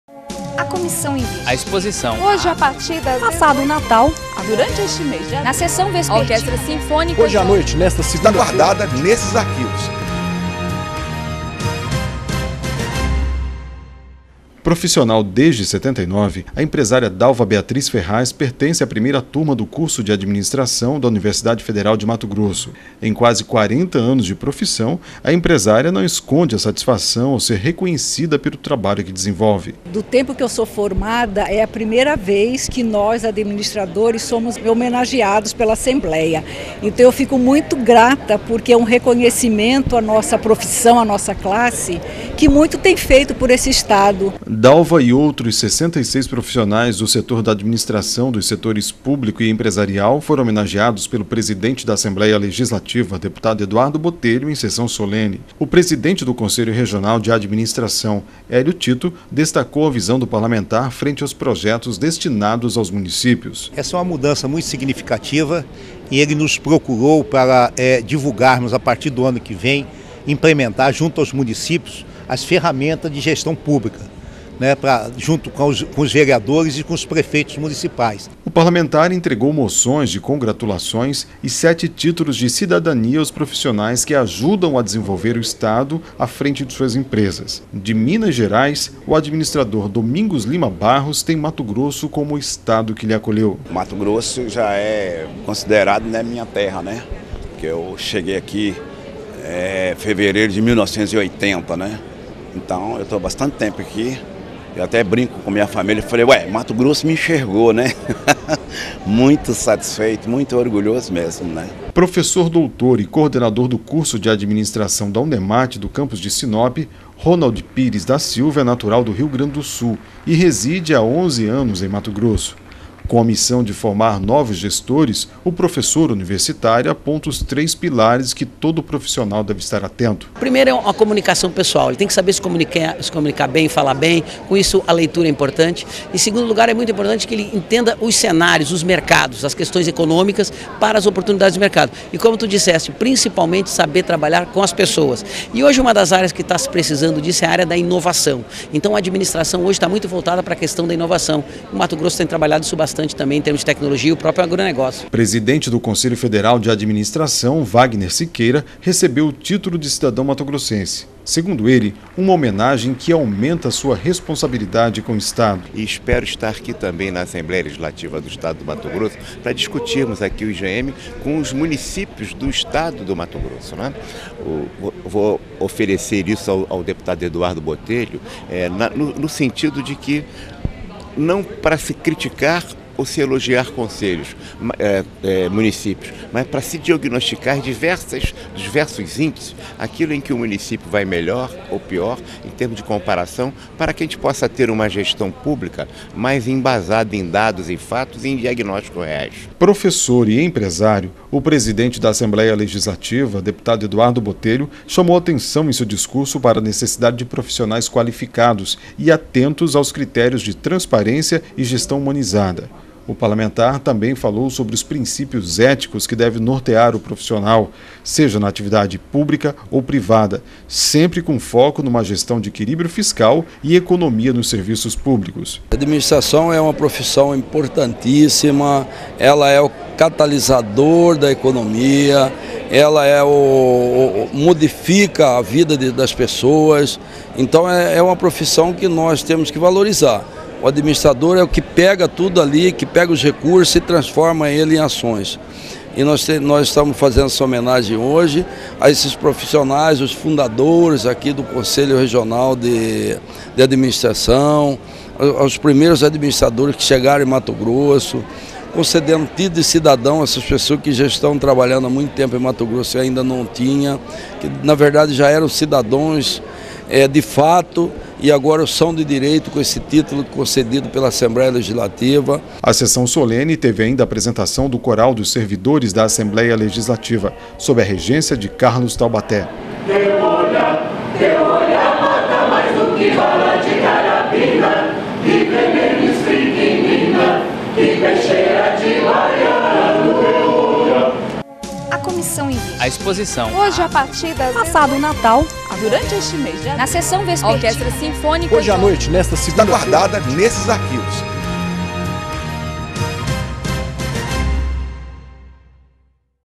ALMT presta homenagem a profissionais de Administração
Sessão solene foi realizada no dia 19 de outubro de 2017.